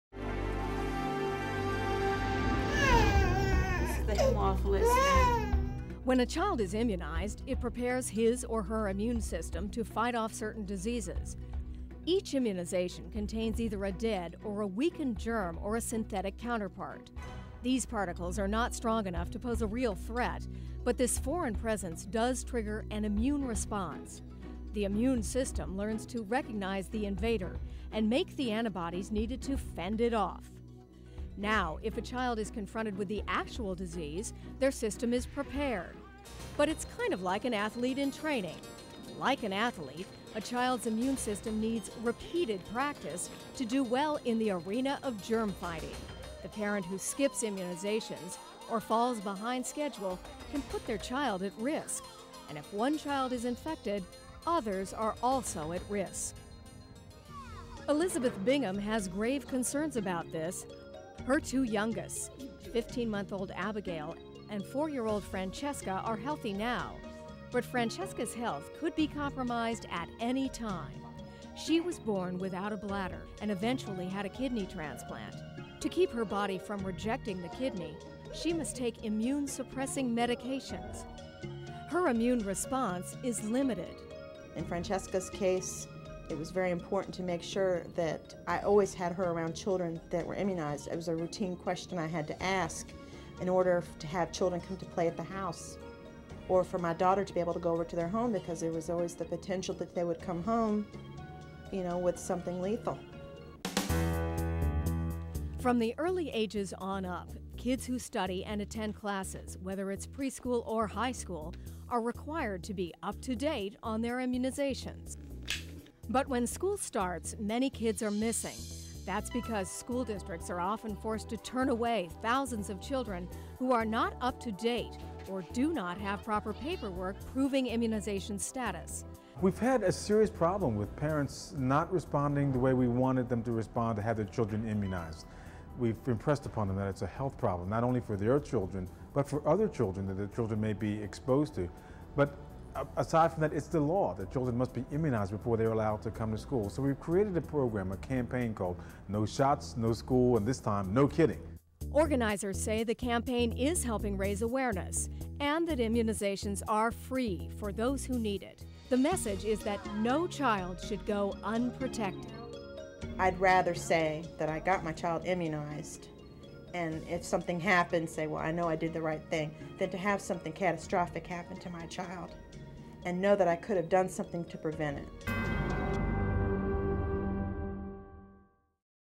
Immunizations for the School Aged Child
The success of modern vaccines is one of the truly extraordinary accomplishments of medical science. In earlier generations many children contracted communicable diseases like polio and whooping cough, frequently with devastating consequences. Some children died; others were left with permanent impairments, perhaps dependent on a wheelchair.